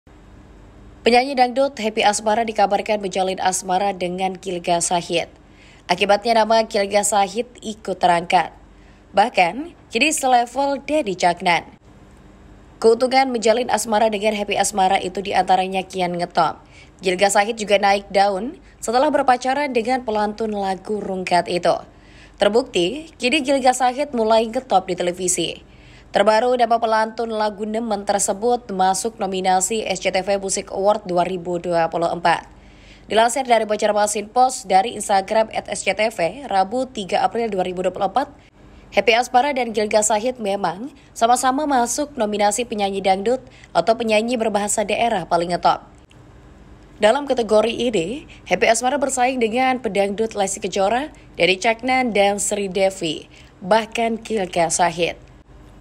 Satu Keuntungan Gilga Sahid Pacari Happy Asmara, Akhirnya Kini Selevel Denny Caknan di TV
BANJARMASINPOST.CO.ID - Penyanyi dangdut Happy Asmara dikabarkan menjalin asmara dengan Gilga Sahid.

Akibatnya, nama Gilga Sahid ikut terangkat. Bahkan, kini selevel Denny Caknan.

Keuntungan menjalin asmara dengan Happy Asmara itu di antaranya kian ngetop.